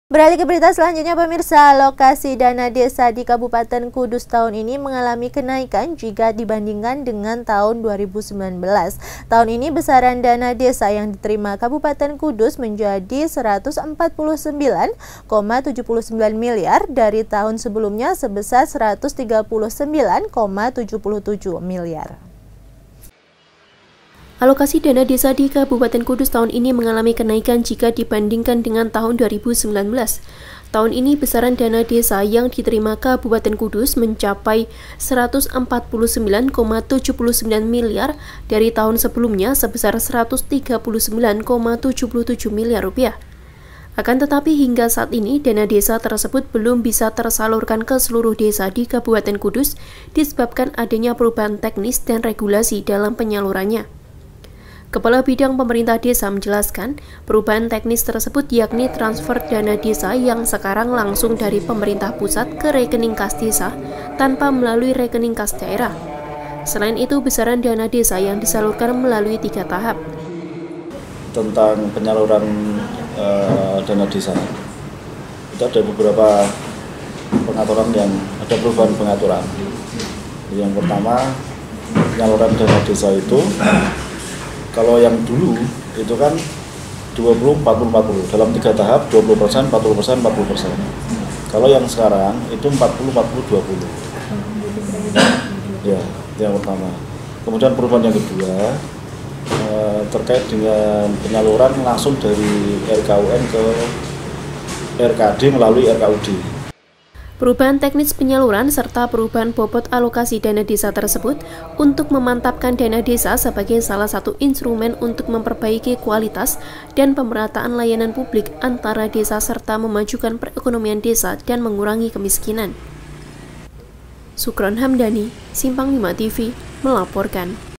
Dana Desa Kudus Naik, Namun Belum Bisa Dicairkan
Alokasi Dana Desa Di Kabupten Kudus Tahun Ini Mengalami Kenaikan Jika Dibandingkan Dengan Tahun 2019. Tahun Ini Besaran Dana Desa Yang Diterima Kabupaten Kudus Menjadi 149,79 Milyar Dari Tahun Sebelumnya Sebesar 139,77 Milyar.